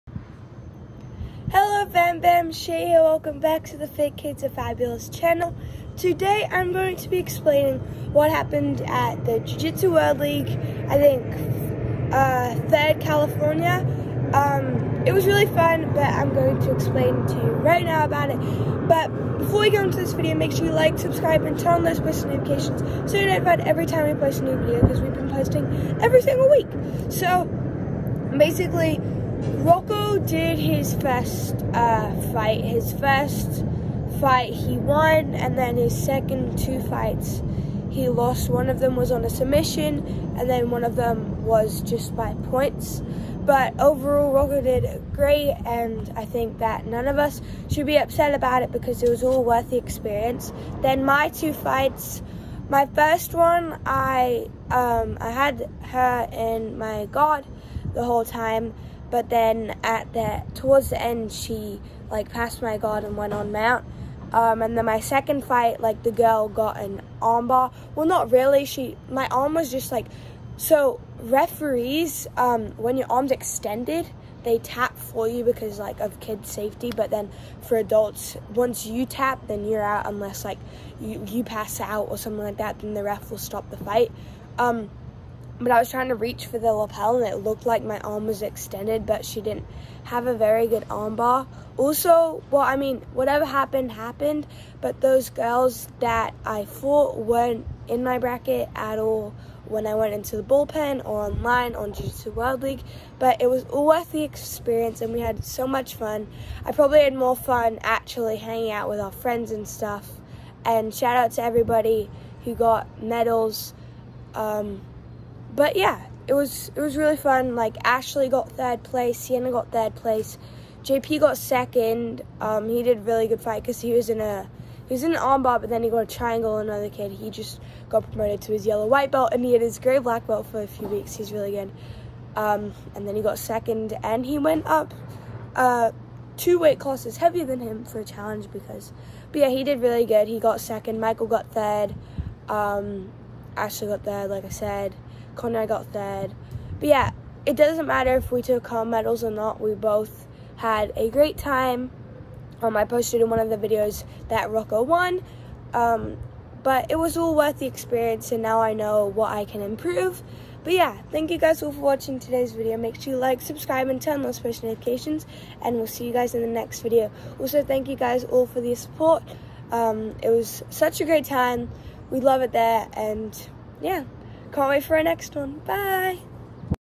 Did we win or learn? Che explains what happened at BJJ California Open #4!
Che explains if it matters to walk home with a medal. But it matters to try your best